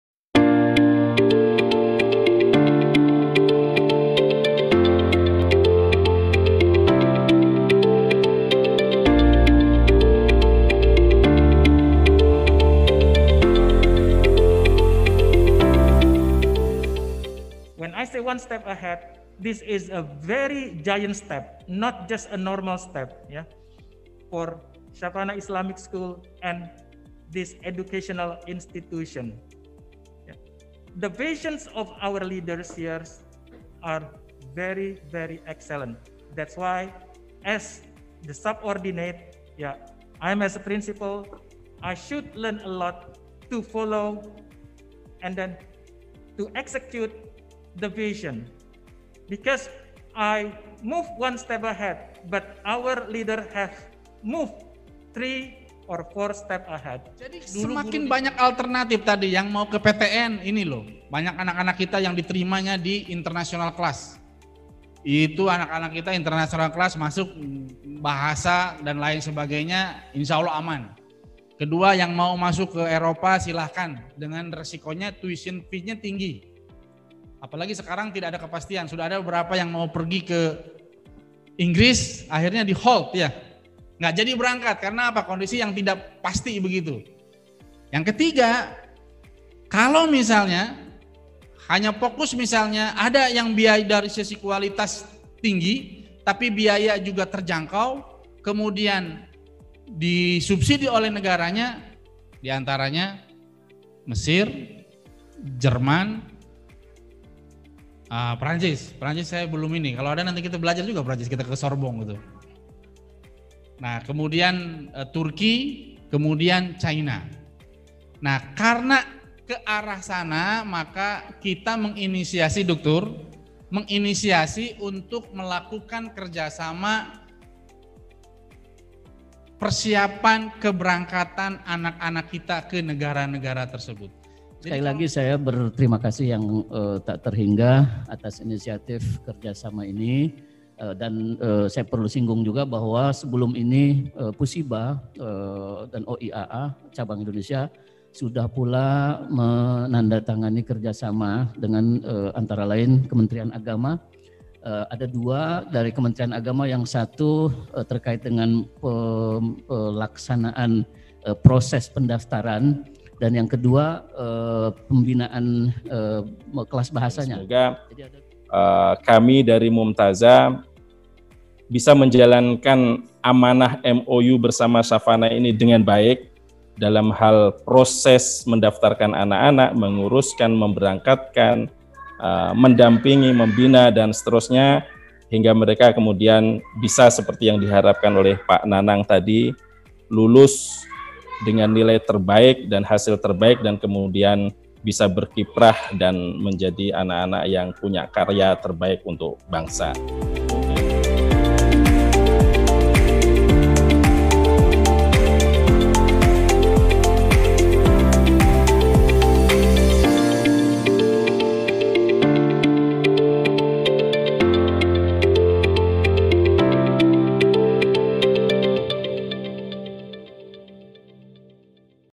PENANDATANGANAN NOTA KESEPAHAMAN BIMBINGAN KULIAH di UNIV. AL AZHAR MESIR dan UNIV. di TURKI
Dalam rangka mempersiapkan pendidikan lanjutan untuk siswa/siswi Upper Secondary (SMA), Syafana Islamic School kembali menjalin kerjasama dengan beberapa lembaga terpercaya yang telah menghantarkan para pelajar Indonesia ke universitas seperti Al Azhar- Mesir dan Turki.

Hal ini dikukuhkan dalam sebuah acara Penandatanganan Nota Kesepahaman atau MoU pada hari Rabu 18 November 2020 antara Syafana Islamic School yang langsung dilakukan oleh Chair Person Hj. Ummu Latifah dan Managing Director H. Nanang Firdaus Masduki, Lc. dengan Pusat Studi Islam dan Bahasa Arab (PUSIBA) oleh H. Muhammad Arifin, Lc. MA sebagai Direktur PUSIBA dan Mumtaza Center oleh H. Ahmad Hafifi, Lc. MA sebagai Pimpinan Mumtaza Center.

Acara yang digelar di Kampus Secondary BSD City dan disiarkan secara virtual ini, dihadiri oleh Pengurus PTA, Menejemen dan Dewan Guru, dengan memperhatikan protokol kesehatan.

Berdasarkan banyak fakta pada catatan pendidikan kita, banyak para siswa sekolah di Indonesia yang saat ini duduk di bangku SMA mengalami kebingungan dalam menentukan pilihan menjelang masuk jenjang perguruan tinggi, hal ini disebabkan minimnya informasi yang mereka dapatkan seputar bangku perkuliahan. Untuk menjawab tantangan ini, Syafana Islamic School terus mengupayakan agar siswa/siswinya dapat menentukan pilihan perguruan tinggi yang sesuai dengan kemampuan dan minat mereka.

Di samping itu juga dengan informasi yang lebih memadai, siswa dapat menentukan perguruan tinggi di luar negeri yang selama ini hanya segelintir pihak yang dapat mengakses kesempatan tersebut. Peluang inilah yang diraih oleh Syafana Islamic School untuk memberikan jalan dan kesempatan dengan lebih mudah melalui kerjasama dengan lembaga pendidikan ternama.

Managing Director Syfana Islamic School. H. Nanang Firdaus Masduki, Lc menyampaikan dalam sambutannya “Syafana mempunyai mimpi besar dalam mempersiapkan para pelajarnya untuk masuk ke perguruan tinggi terbaik di dalam maupun di luar negeri, berdasarkan pada sebaran alumni Syafana saat ini yang sudah diterima di berbagai PTN dan masuk di berbagai kelas internasional, juga beberapa alumni yang sudah masuk universitas luar negeri seperti Jerman dan Malaysia”.

Berbekal kemampuan bahasa yang baik, Syafana mempersiapkan siswa/siswi untuk masuk ke universitas di berbagai negara dengan kualitas pendidikan terbaik dan biaya yang relatif terjangkau.

Ust. Nanang Firdaus melanjutkan, “Melalui kerjasama ini kita membuka kesempatan yang lebih luas untuk siswa/siswi Syafana untuk menimba ilmu di luar negeri sehingga ke depan mereka bisa kembali ke tanah air dan membangun Indonesia menjadi lebih baik”.

Alhamdulillah, tahun ini salah satu siswa Syafana Donald Tanri Ramadhan (angkatan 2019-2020) berhasil lolos seleksi untuk masuk ke Universitas Istanbul berkat kerjasama Syafana dengan Mumtaza Center.

Kerjasama ini menandai terwujudnya komitmen Syafana untuk terus meningkatkan layanan kepada siswa/siswinya dalam menentukan pilihan mereka masuk perguruan tinggi. Seperti yang disampaikan oleh Ust. Sainur Rahim, M.Pd selaku Kepala Sekolah SMA Syafana, “Kerjasama ini merupakan sebuah langkah besar untuk Syafana ke depan sekaligus memberikan kesempatan bagi seluruh siswa dan orang tua untuk menentukan masa depan calon pemimpin bangsa”.